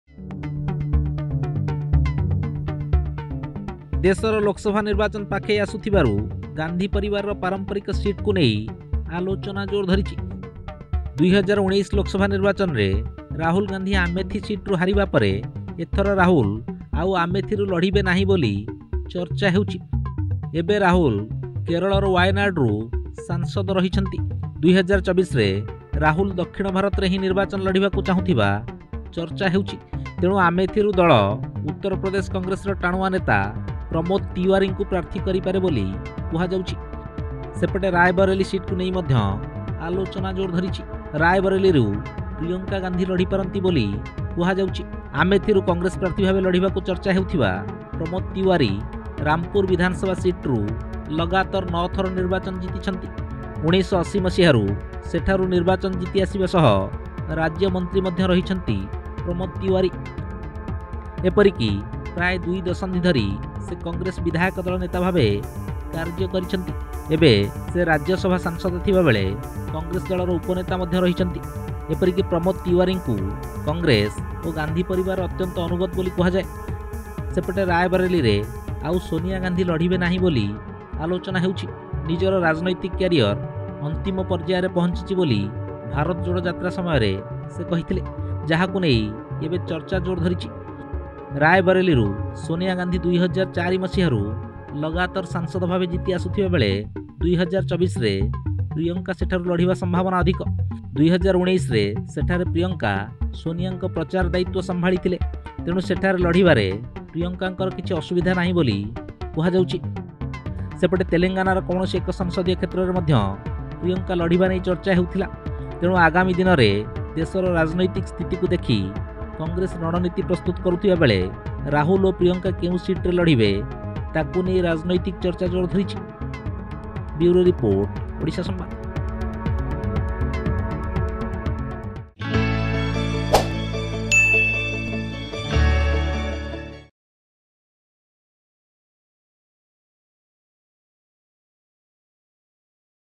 Priyanka May Contest Election From Raebareli, Sonia Will no Longer Contest!
ରାୟବରେଲି ସିଟକୁ ନେଇ ଆଲୋଚନା ଜୋର ଧରିଛି। ରାୟବରେଲିରୁ ପ୍ରିୟଙ୍କା ଲଢ଼ିପାରନ୍ତି ବୋଲି କୁହାଯାଉଛି।